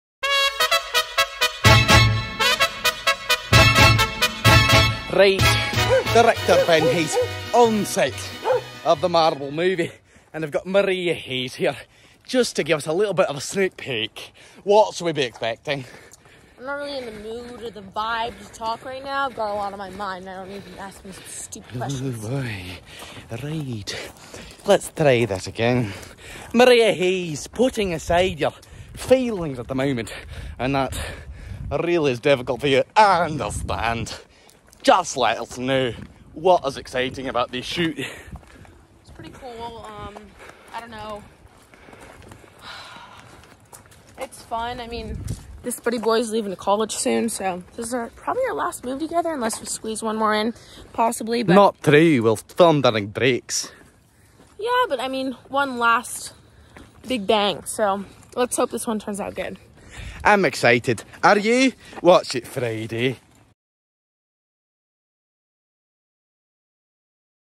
Nonthian Films Update #13: The Marble Movie
Accompany Ben and Marea Hayes on set as they discuss their latest movie.